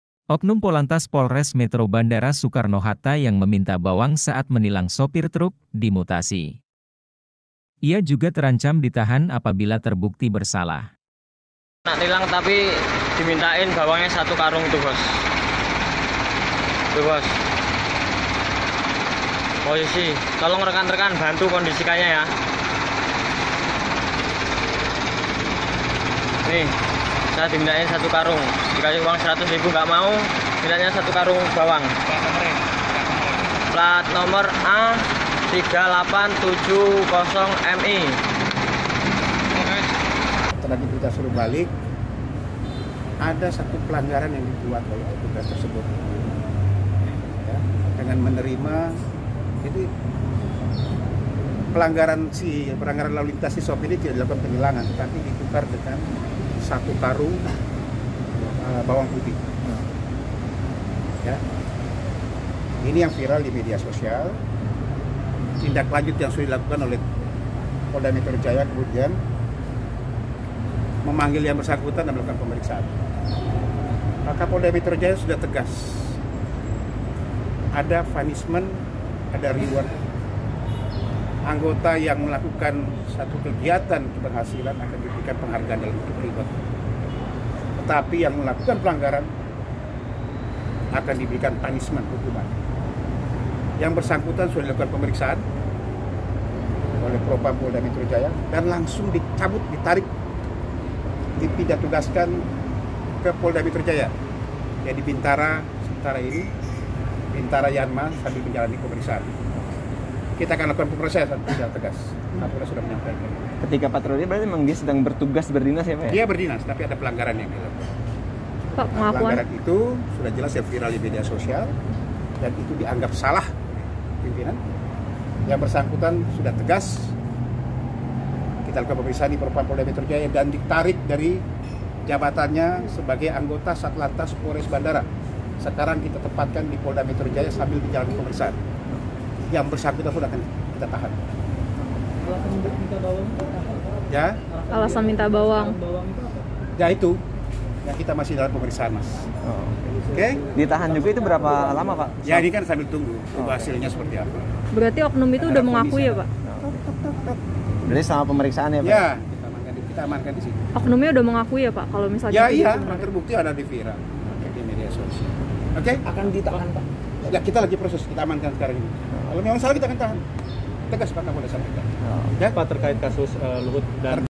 Polantas Bandara Soetta Minta Bawang Sekarung Dimutasi
Oknum Polantas Polres Metro Bandara Soekarno-Hatta yang meminta bawang saat menilang pengendara sopir truk dimutasi. Ia juga terancam ditahan apabila terbukti bersalah.

Kabid Humas Polda Metro Jaya Kombes Pol Yusri Yunus mengatakan bahwa pihaknya telah memeriksa oknum Polantas inisial Aiptu PDH.

Kata Yusri, hasil pemeriksaan sementara, Aiptu PDH mengakui perbuatannya telah menukar surat tilang dengan satu karung bawang.

"Dimana saat ada pelanggaran lalu lintas tidak dilakukan penilangan, tapi ditukar dengan satu karung bawang putih,"